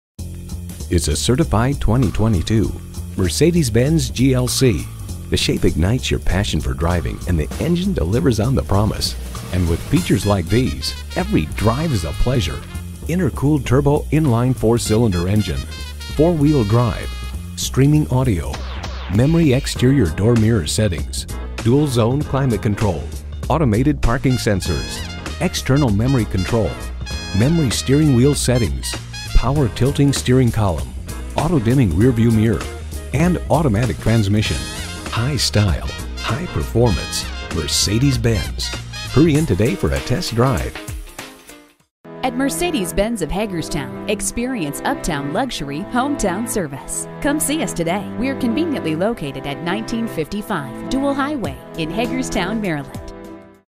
Certified 2022 Mercedes-Benz GLC Martinsburg, WV Hagerstown, MD M8515X - SOLD
Year: 2022
Make: Mercedes-Benz
Model: GLC
Trim: GLC 300 4MATIC SUV
Engine: 4 2.0 L
Transmission: Automatic
Color: Black
Interior: Silk Beige/Black
Mileage: 11249
Stock #: M8515X
VIN: W1N0G8EB9NV334275
Boasts 28 Highway MPG and 21 City MPG! This Mercedes-Benz GLC delivers a Intercooled Turbo Premium Unleaded I-4 2.0 L/121 engine powering this Automatic transmission. HEATED STEERING WHEEL, WHEELS: 8J X 18 ET 38 SPLIT 5-SPOKE (STD), SURROUND VIEW SYSTEM.* This Mercedes-Benz GLC Features the Following Options *MULTIMEDIA PACKAGE LITE -inc: Mercedes-Benz Navigation, Live Traffic, Navigation Services, Speed Limit Assist, DRIVER ASSISTANCE PACKAGE -inc: active speed limit assist, route-based speed adaptation, active lane change assist and Active Emergency Stop Assist, PRE-SAFE PLUS, DISTRONIC PLUS w/Steering Assist, stop and go pilot, PRE-SAFE Brake w/Pedestrian Recognition, BAS PLUS w/Cross-Traffic Assist, Active Blind Spot Assist, Active Lane Keeping Assist, Driver Assistance Package Plus , SILK BEIGE/BLACK, MB-TEX UPHOLSTERY, PORCELAIN FABRIC HEADLINER, NATURAL GRAIN WALNUT WOOD TRIM, EMERGENCY SPARE WHEEL, CLASS II TRAILER HITCH, BLACK, ALL-SEASON BLACK RUBBER FLOORMATS (PACK OF 4), Window Grid Antenna.*Visit our **Contact us: Fill out the contact form or call us at OR visit our website for Express Purchase or Express Test Drive.

Address:
1955 Dual Highway